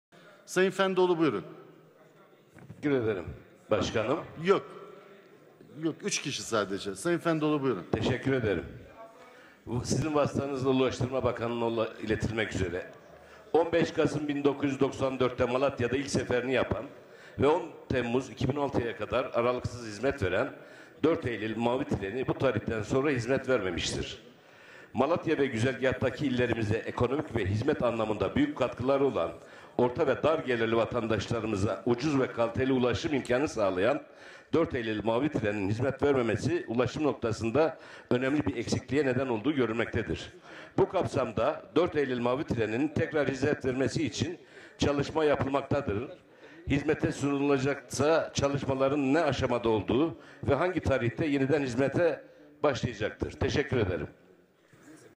4 Eylül Mavi Treni Tekrar Sefere Başlamalıdır
4 Eylül Mavi Treni Tekrar Sefere Başlamalıdır
MHP MALATYA MİLLETVEKİLİ MEHMET CELAL FENDOĞLU